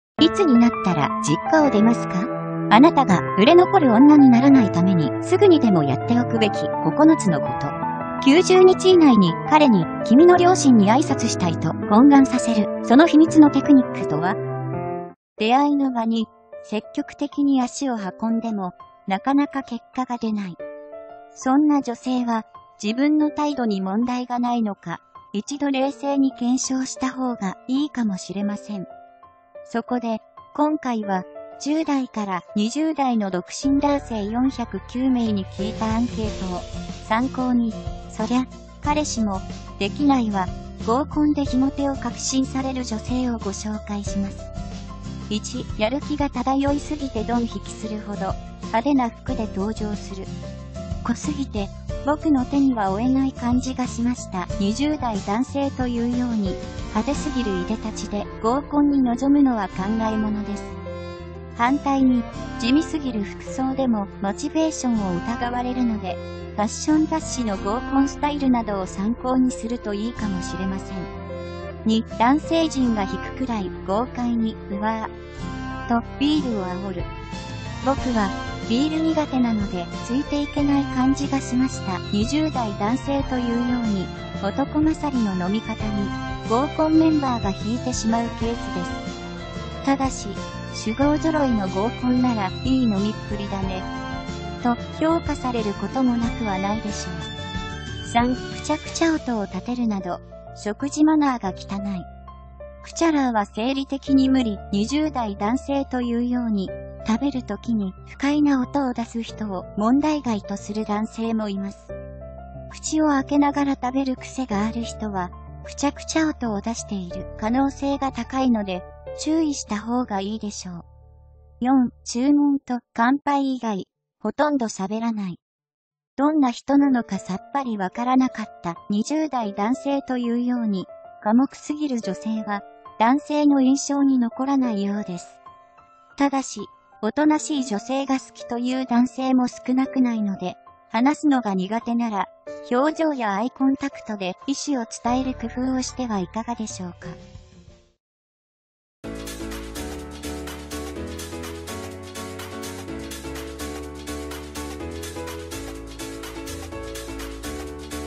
①そりゃ彼氏もできないわ…合コンで非モテを確信される女性
彼氏が欲しい！大好きな彼を振り向かせたい！
復縁したい！恋愛の心理学やエクササイズなど無料配信中！

出会いの場に積極的に足を運んでも、なかなか結果が出ない…。そんな女性は、自分の態度に問題がないのか、一度冷静に検証したほうがいいかもしれません。そこで今回は10代から20代の独身男性409名に聞いたアンケートを参考に「そりゃ彼氏もできないわ…合コンで非モテを確信される女性」をご紹介します。